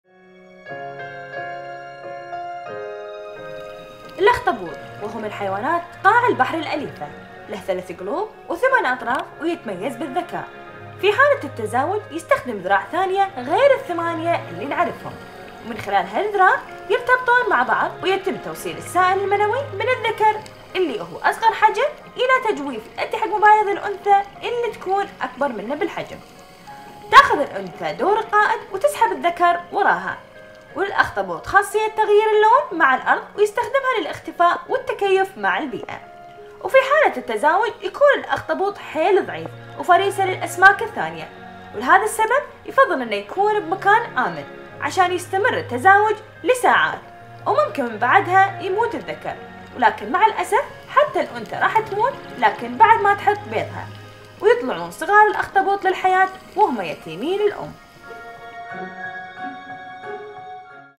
OCTOPUS MATING || تزاوج الاخطبوط
الاخطبوط وهو من حيوانات قاع البحر الاليفه
له ٣ قلوب و ٨ اطراف و يتميز بالذكاء
في حاله التزاوج يستخدم ذراع اخرى غير عن ال ٨ آطراف المعروفه ، و من خلال هذه الذراع يتم التلقيح ( فيتم توصيل السائل المنوي من الذكر و الذي يكون اصغر حجما الى تجويف يؤدي الى مبايض الانثى والتي تكون آكبر من الذكر حجما )
تأخذ الانثى دور القائد و تسحب الذكر وراءها، وللاخطبوط خاصيه تغيير اللون مع الارض و يستخدمها للاختفاء و التكييف مع البيئه.
وفي حالة التزاوج يكون الاخطبوط ضعيف جدا و فريسة للاسماك الثانية و لهذا السبب يفضل ان يكون في مكان آمن ليستمر التزاوج لساعات. و ممكن بعةها يموت الذكر ،، و مع الاسف فان الانثى ايضا سوف تموت عند وضع البيض . و يخرج صغار الاخطبوط للحياه و هم يتيمين الام.